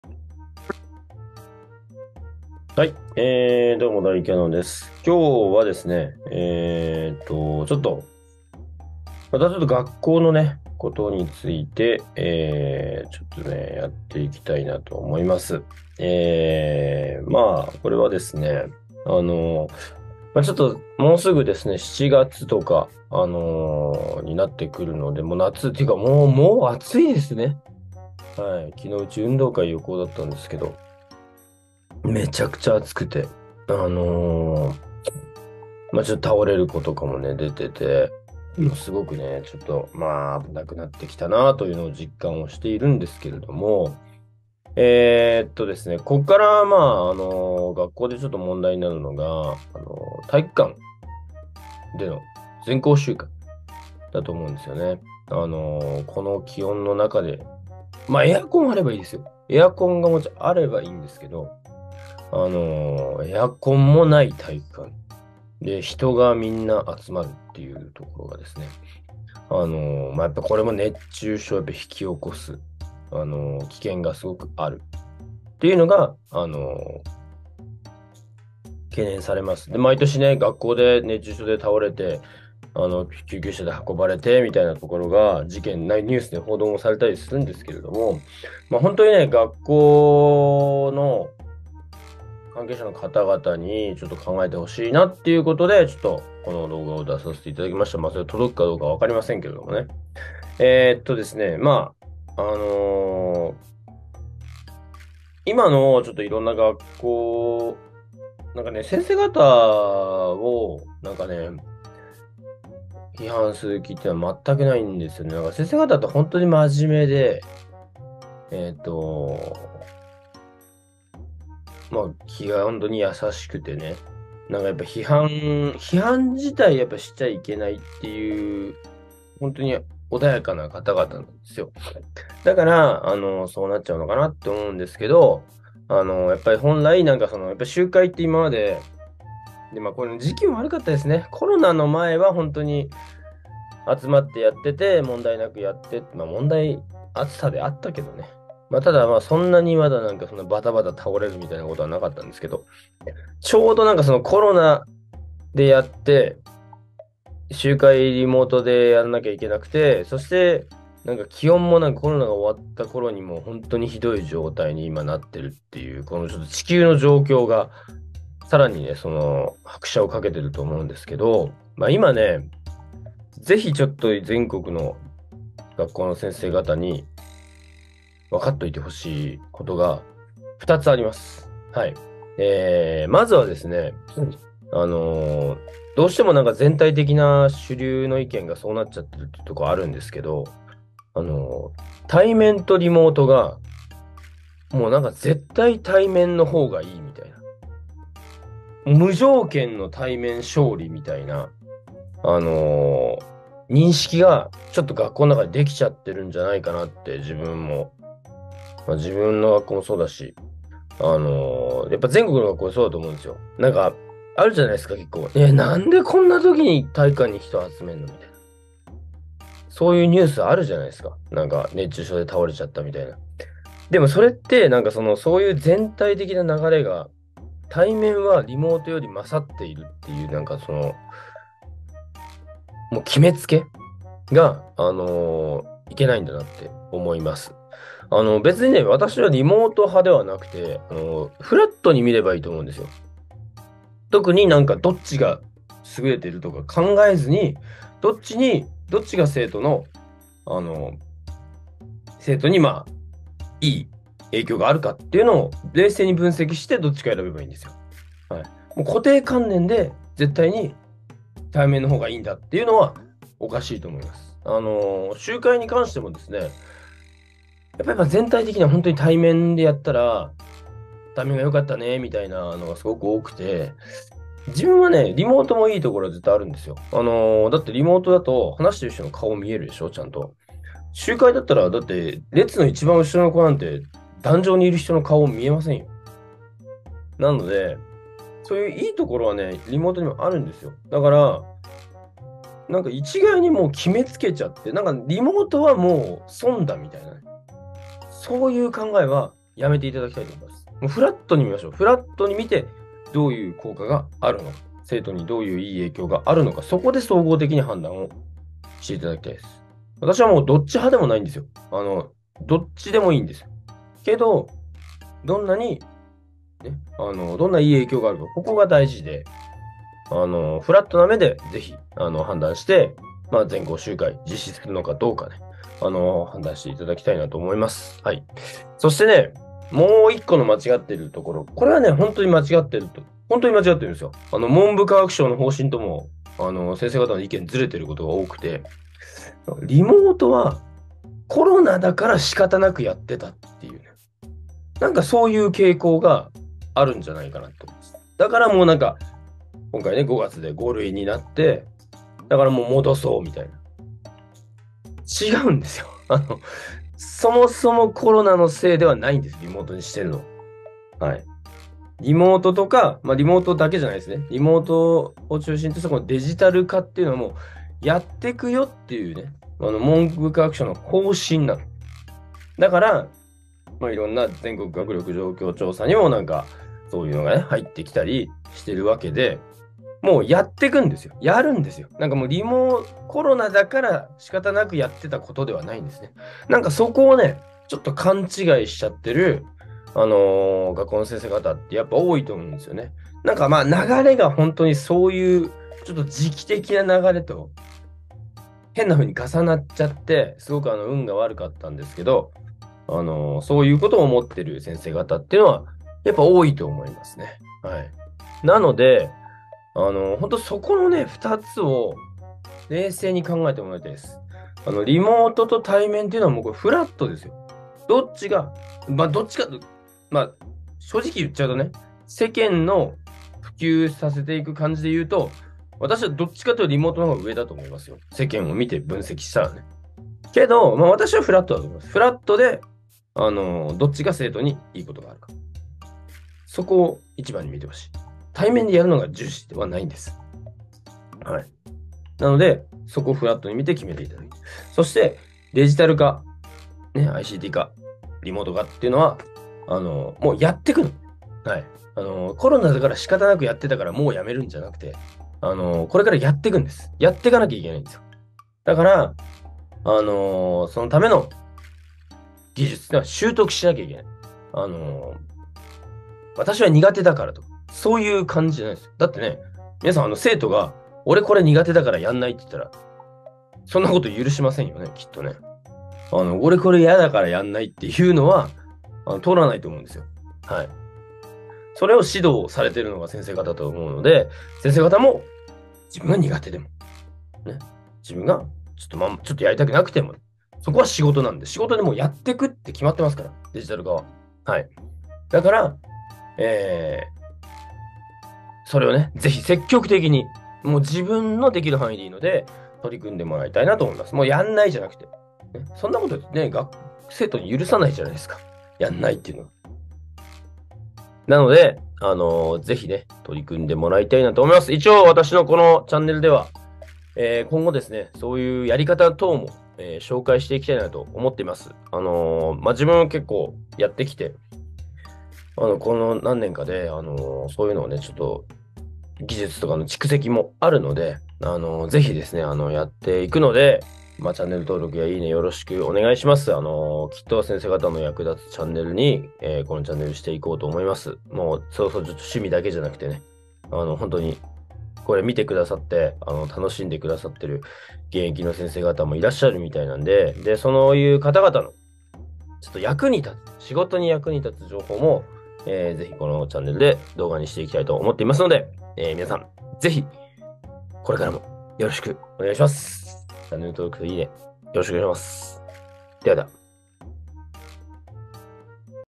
対面🆚リモート‼︎学校が立たされる岐路
ここ数年、学校の教育活動での熱中症などな増えてきている。
しかし、学校はなぜか生徒を集めようとする。

それはなぜなのか？
リモート集会ではなぜいけないのか？
そんな先生方はどんな思いで対面集会にこだわるのか？